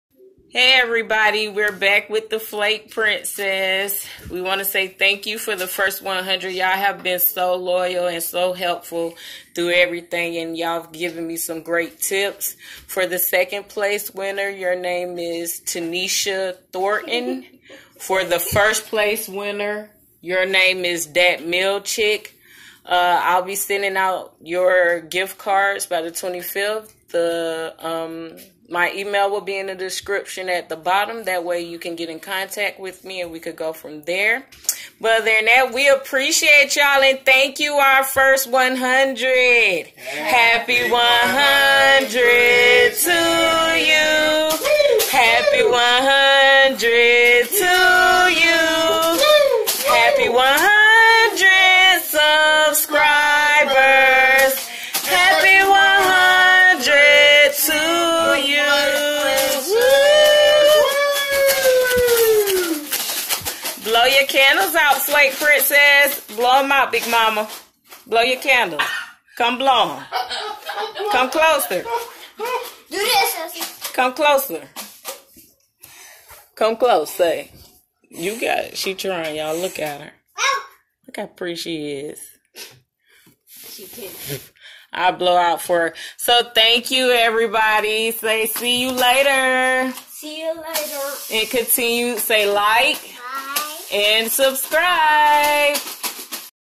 Celebrating Her First 100SUB's
Thank you Everyone we appreciate all of you and all the advice 😘😘😘 big mama had an healthy alternative ice cream, uhhmm hmmm because you know sister girl still on her diet change😂😂😂 (1stDatmilchick) (2ndTenesha Thorton) Flake Princess randomly pick these two.